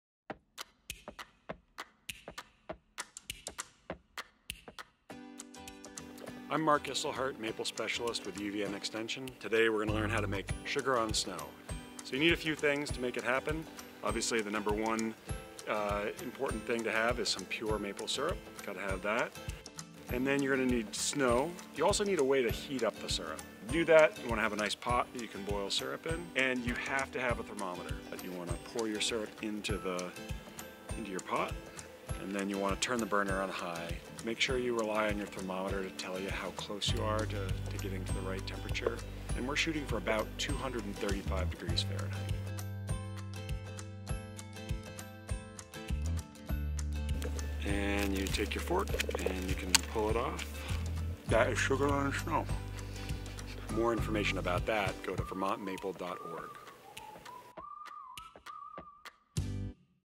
UVM 101: How to Make "Sugar on Snow"
University of Vermont Extension Maple Research Specialist Mark Isselhardt walks you through how to make the springtime classic maple treat "sugar on snow".